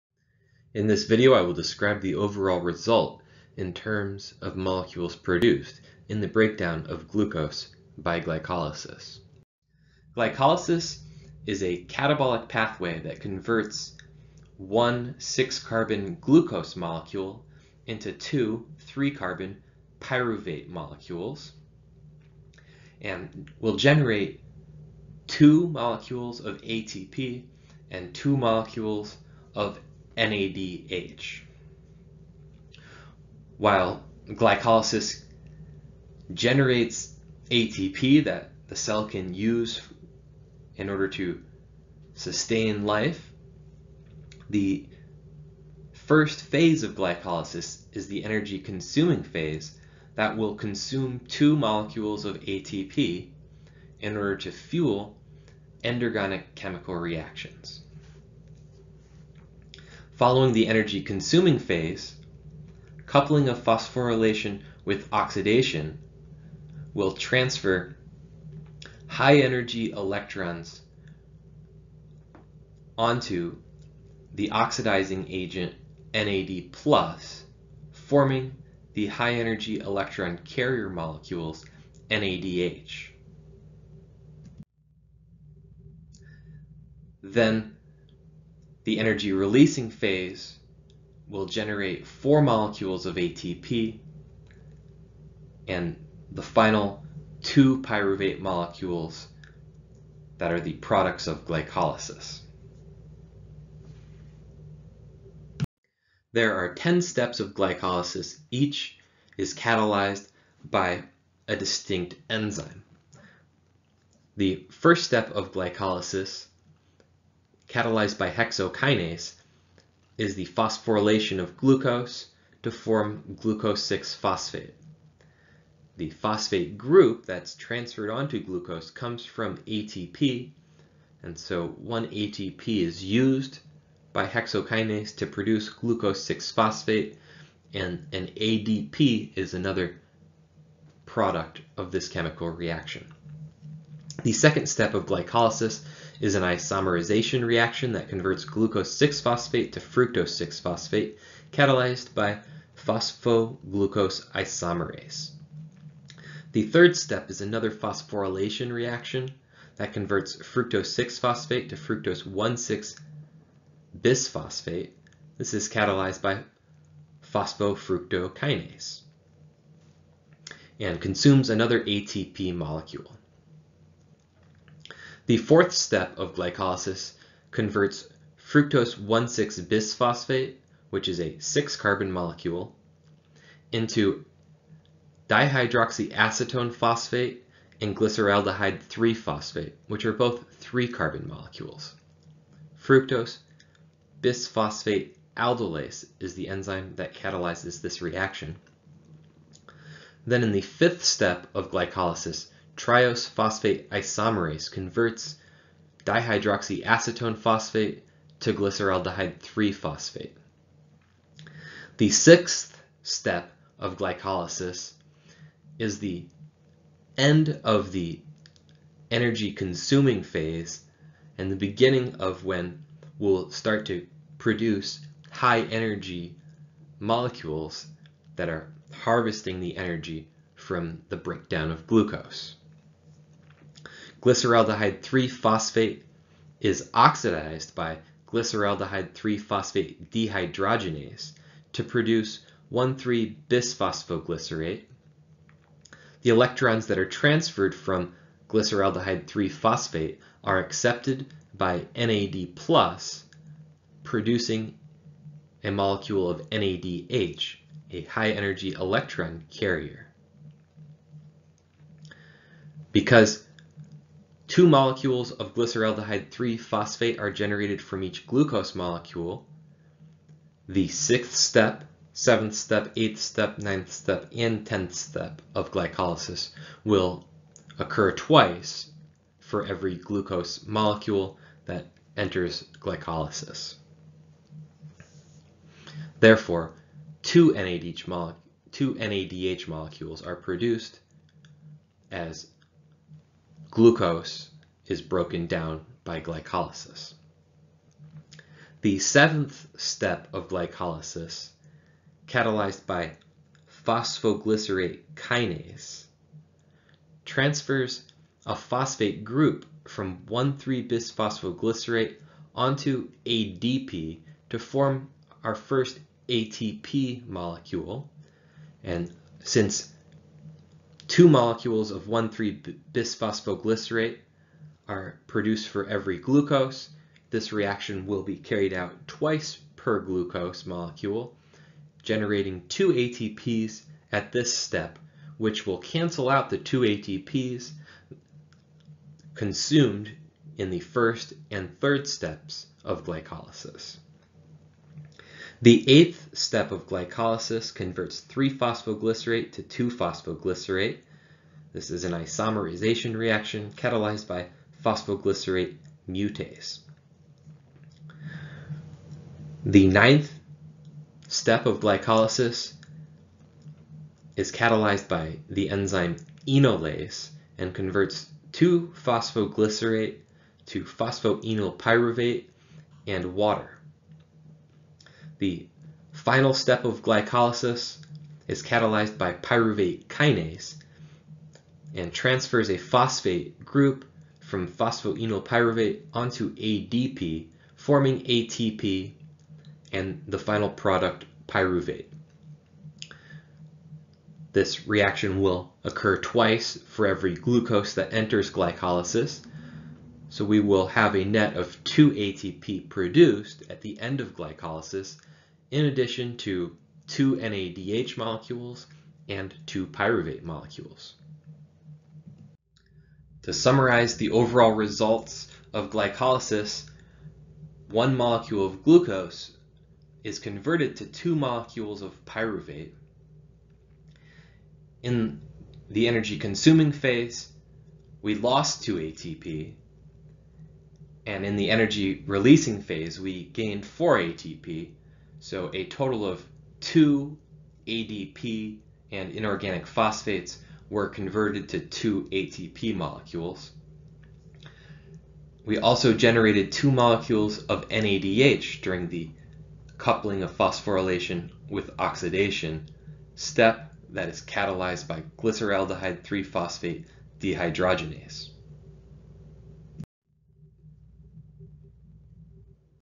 Cellular Respiration part 2
Objective: Describe the overall result in terms of molecules produced in the breakdown of glucose by glycolysis.